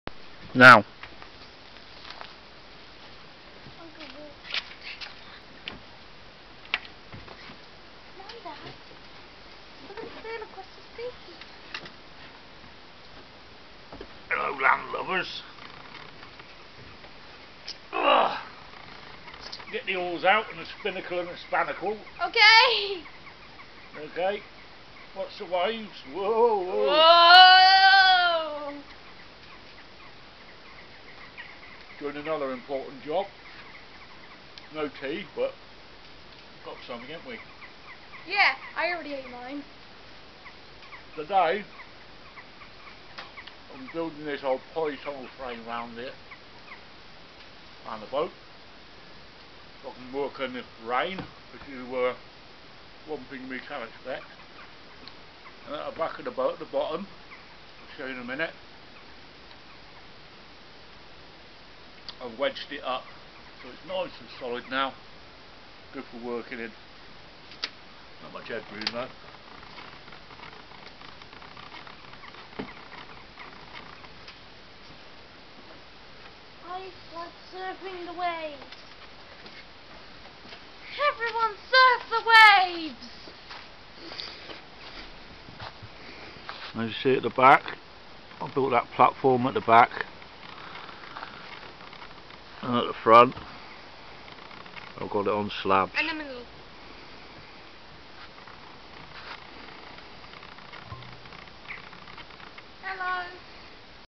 "1961 Yachting world Dayboat" episode 3, part 1
This is the second video of the series "1961 Yachting world Dayboat" renovating series.

This is the video that I start on the boat.
comment and ask questions for me to answer.